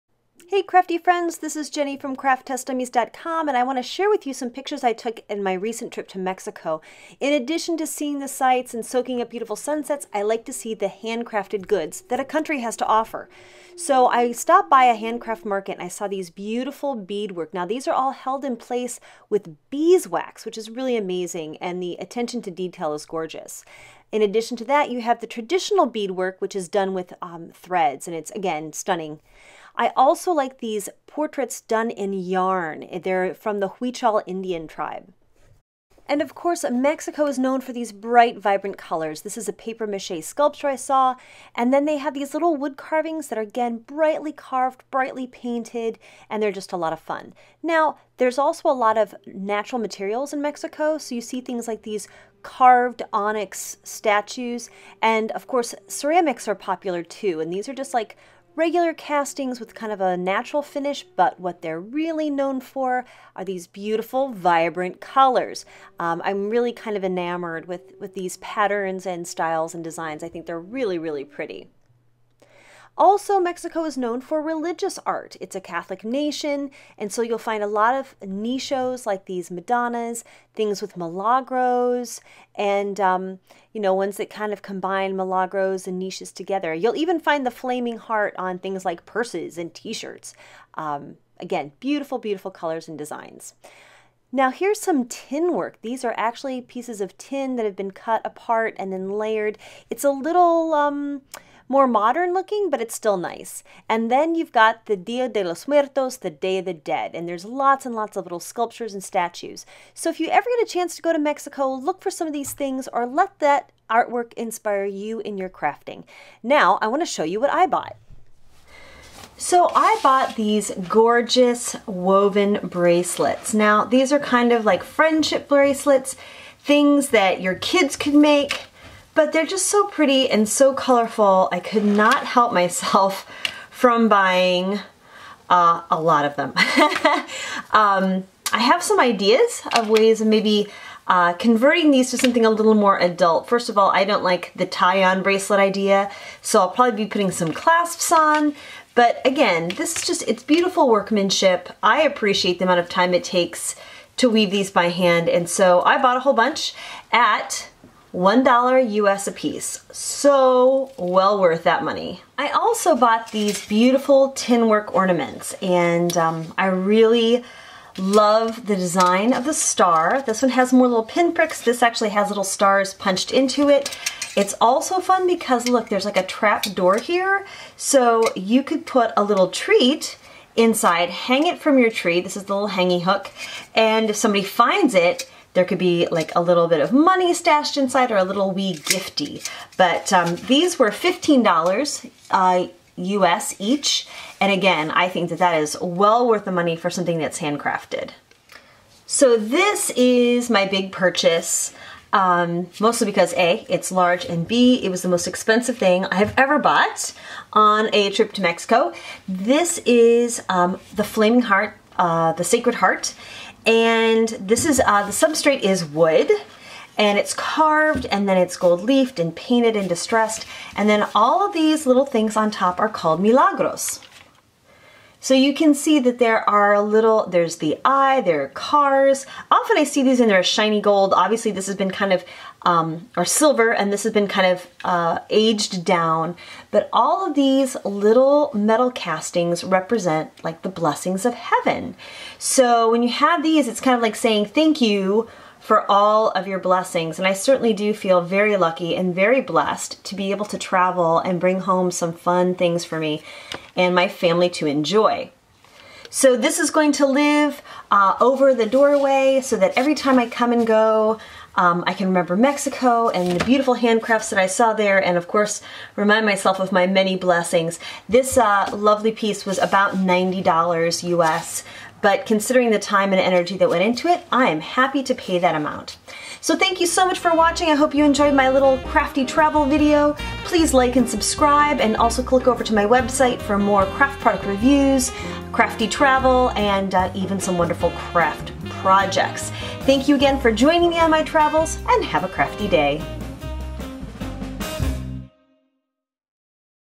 Crafty Travel: Hand Crafts in Mexico!
I continue my Crafty Travel with a trip to sunny MEXICO! I took in the Museum of the Maya and also scoured the markets for traditional Hand Crafts. So much inspiration!